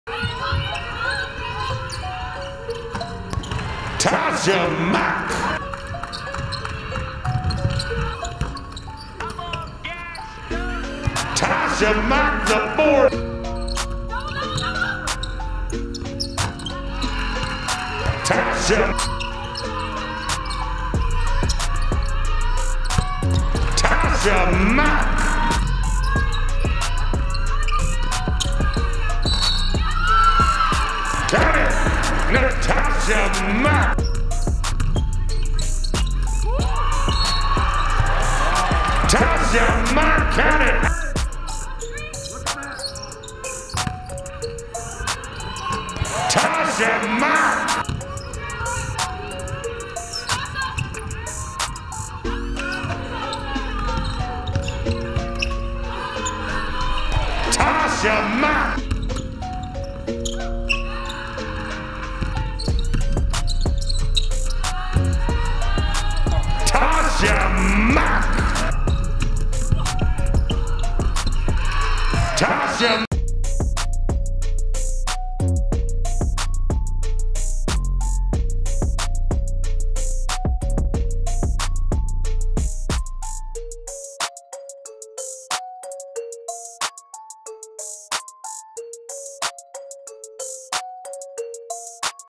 Tasha Time | Natasha Mack vs Kansas State
Natasha Mack leads the way with another dominant performance for the Cowgirls en route to victory over Kansas State